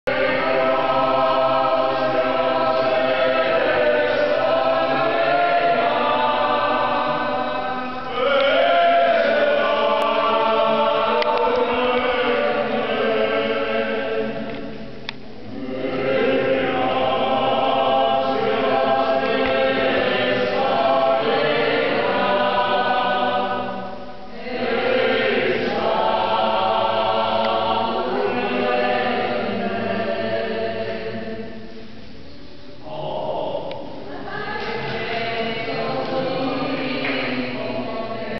St.Michael's Cave - Gibraltar
Impromptu outburst of singing from some tourists in St.Michael's Cave, Gibraltar